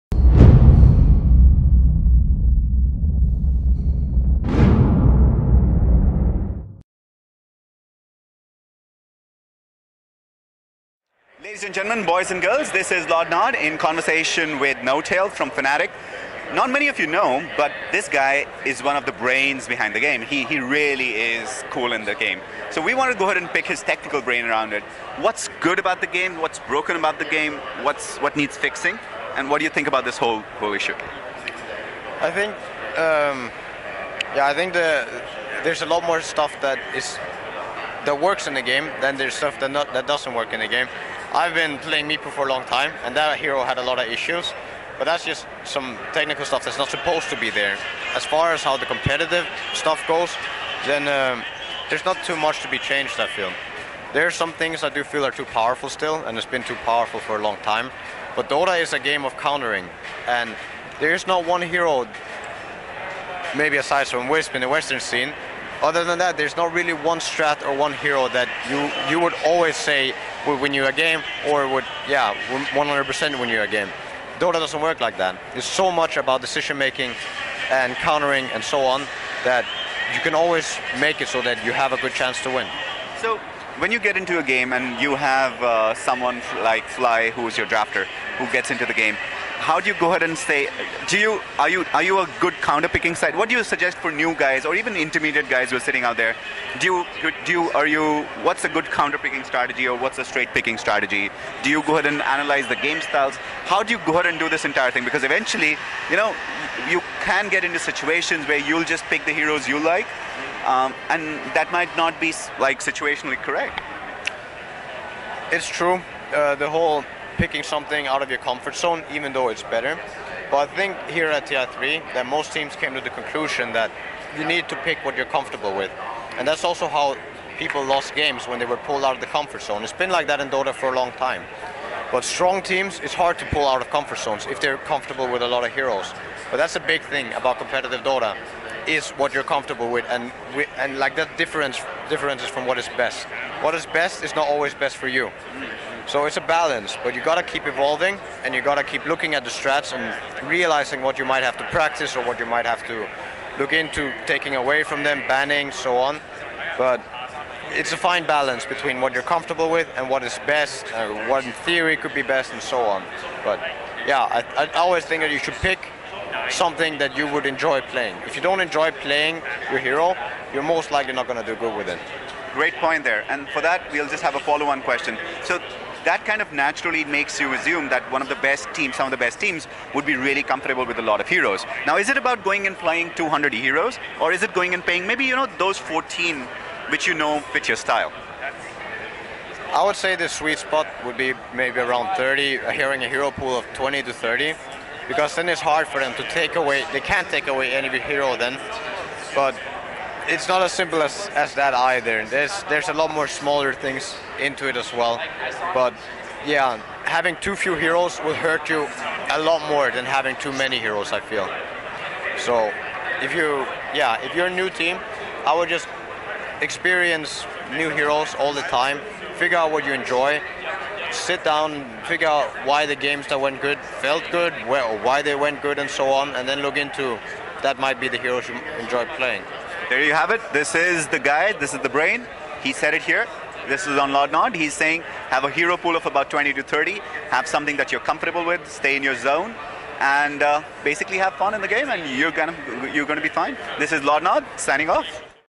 DOTA2 TI3 Tangos - Fnatic.Notail - Talking about the game
Lordnod gets a hold of Fnatic N0tail at the International 3 and they chat about the technical aspect of Dota 2 and what changes he recommends for Dota 2